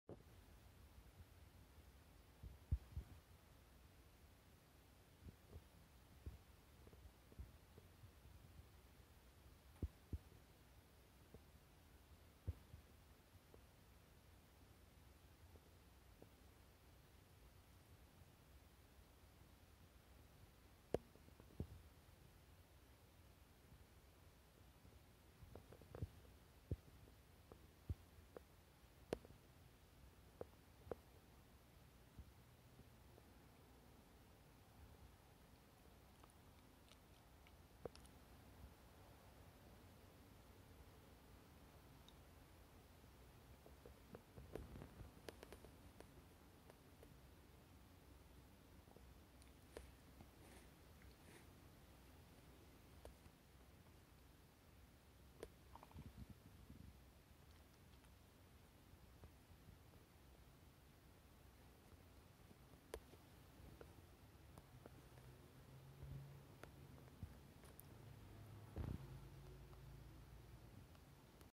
毛づくろいしてあげる猫 １２
ソファで仲良く寛ぐ猫達です。ブログ『ブルー・シュシュ＆リリィ～ソマノル猫日記～』